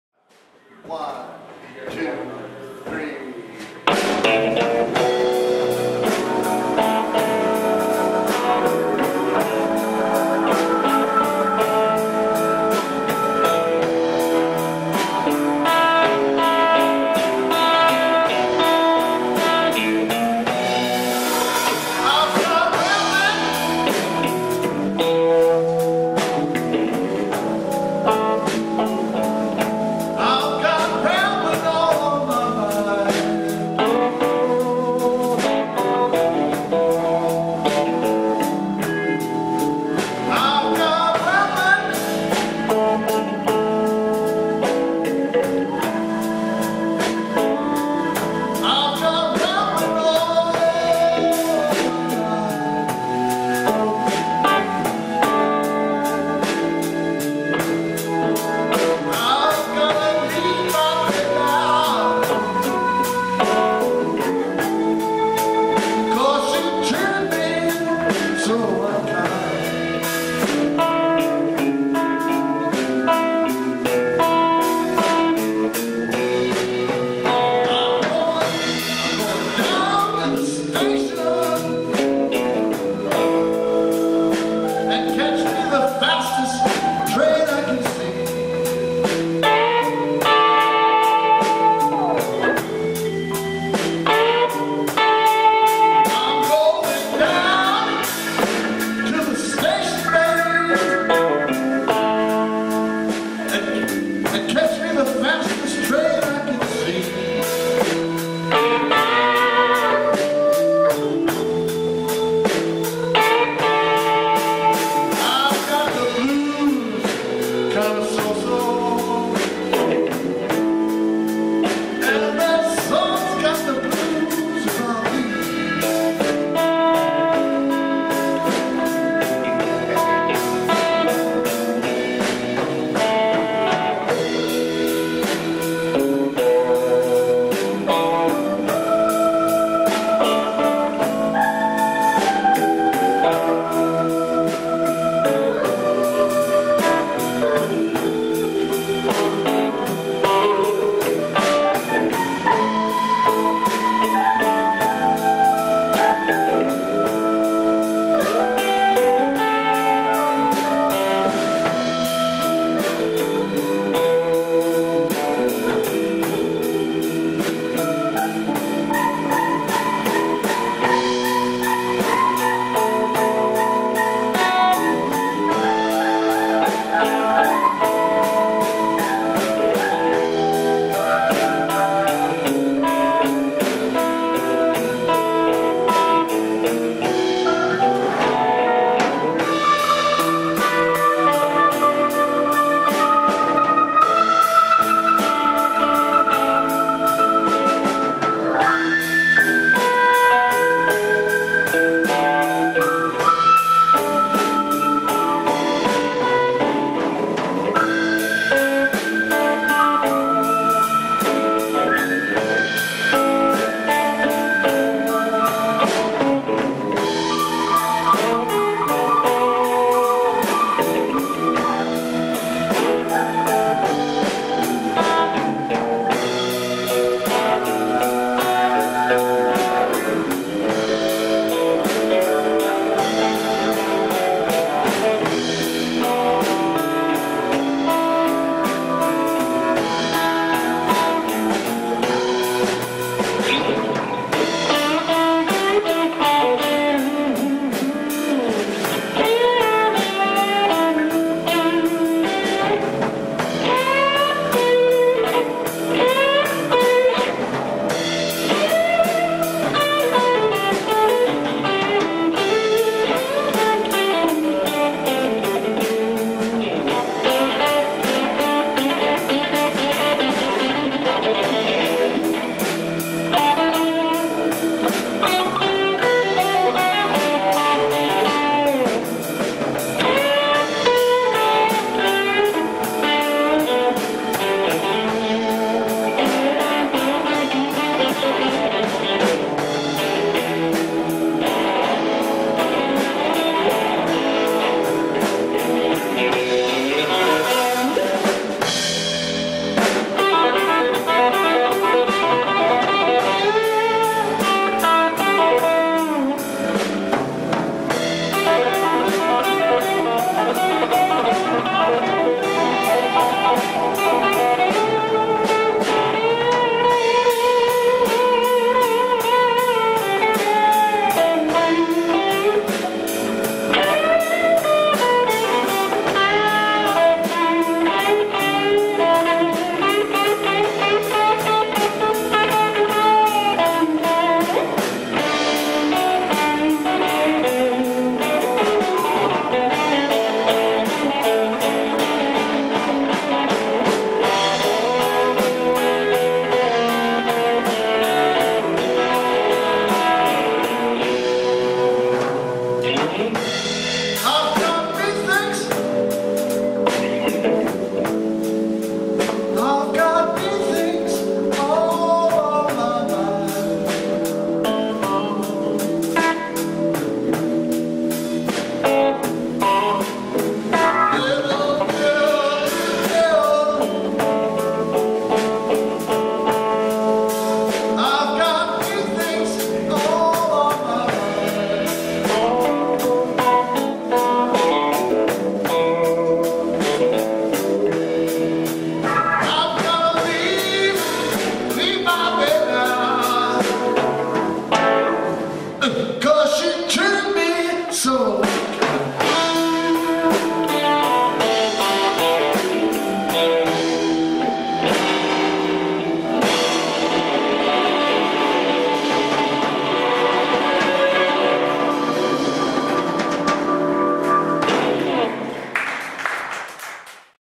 PGvideoAZ Rhythm Street Rambling.flv
Rhythm Street Live Performance for Ballare!
Olive Branch Mediterranean Fusion Grill
Tempe, AZ
Recorded and Produced by PG Video Productions